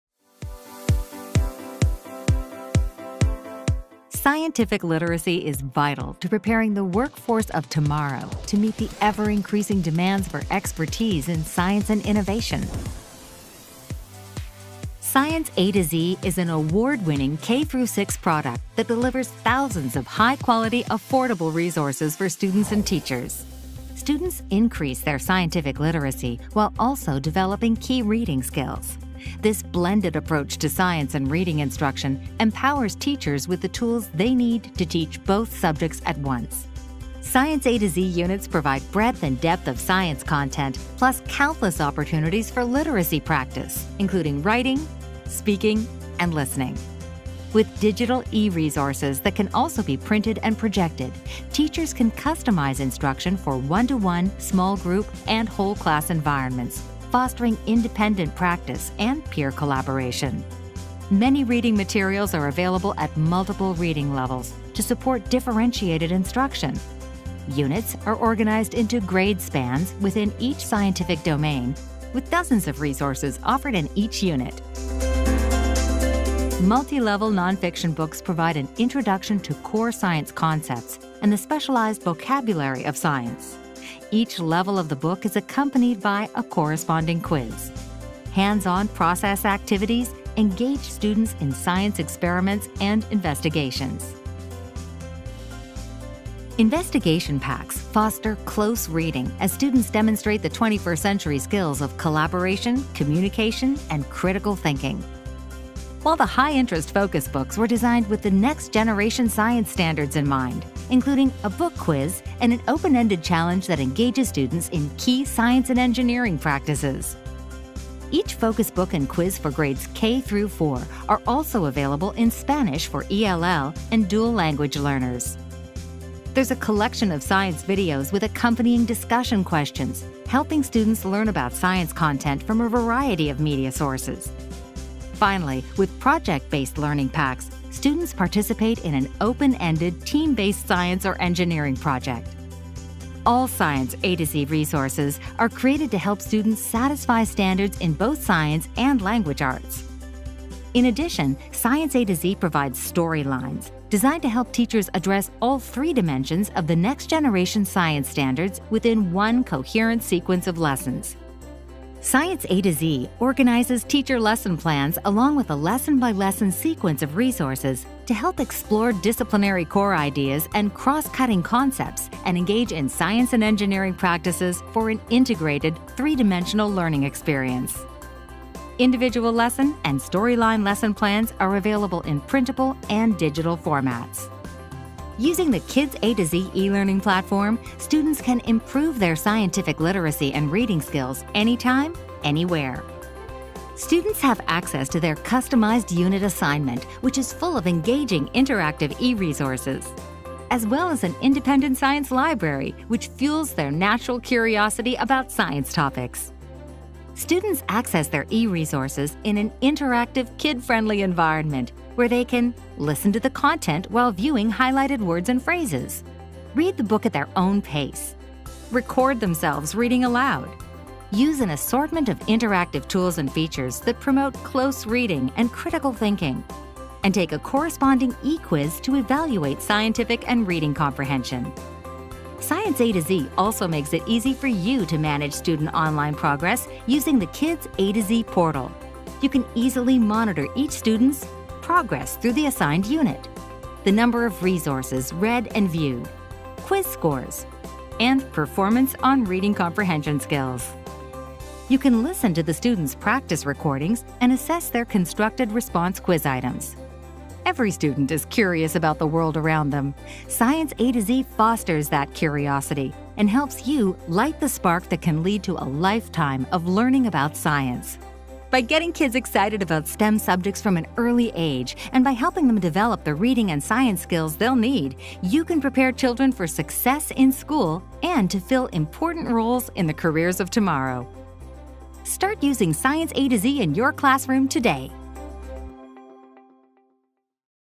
Science A-Z Overview
Science A-Z is an award-winning learning solution that provides resources and tools that integrate science and reading into one captivating curriculum that develops students’ ELA skills and scientific literacy. Watch this video to see how to use Science A-Z resources to easily differentiate instruction and provide hands-on learning experiences.